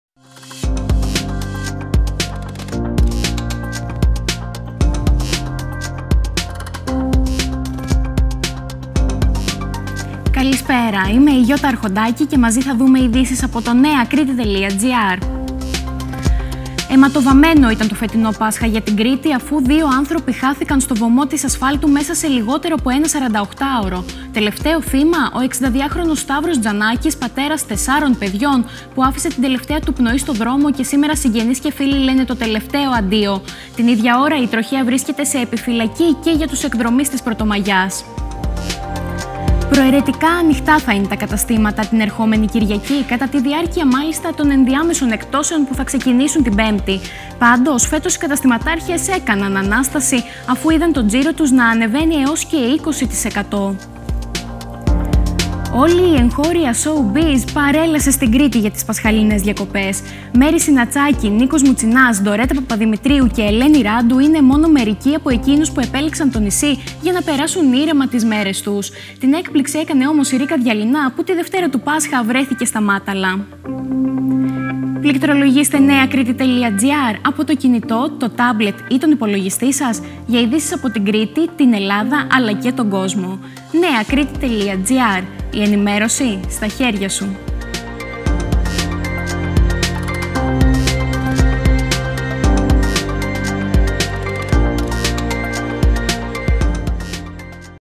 Αποχαιρετούν σήμερα τον 62χρονο πατέρα που σκοτώθηκε σε τροχαίο
Αιματοβαμμένο ήταν το φετινό Πάσχα για την Κρήτη, αφού δύο άνθρωποι χάθηκαν στο βωμό της ασφάλτου μέσα σε λιγότερο από ένα 48ωρο. Τελευταίο θύμα ο 62χρονος Σταύρος Τζανάκης, πατέρας τεσσάρων παιδιών που άφησε την τελευταία του πνοή στο δρόμο και σήμερα συγγενείς και φίλοι λένε το τελευταίο αντίο. Την ίδια ώρα, η Τροχαία βρίσκεται σε επιφυλακή και για τους εκδρομείς της Πρωτομαγιάς.